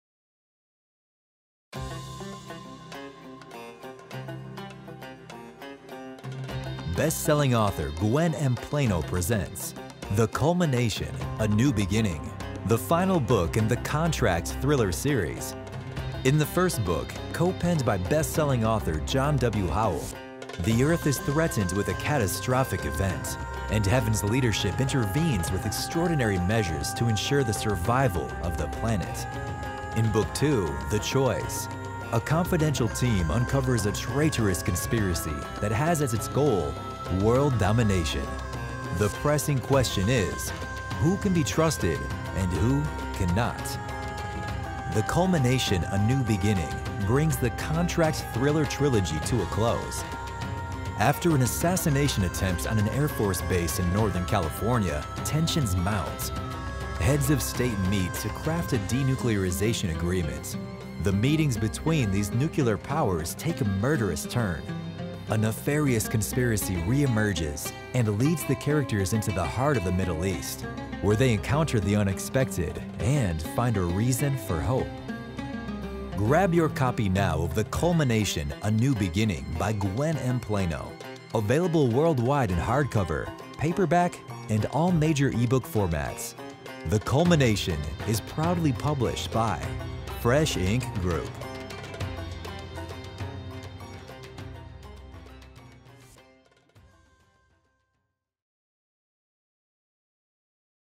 The Culmination: a new beginning by Gwen M. Plano
The Culmination, a new beginning is the third book in The Contract thriller series. After an assassination attempt on an Air Force base in northern California, tensions mount. Heads of state meet to craft a denuclearization agreement. The meetings between these nuclear powers take a murderous turn. A nefarious conspiracy re-emerges and leads the characters into the heart of the Middle East, where they encounter the unexpected and find a reason for hope.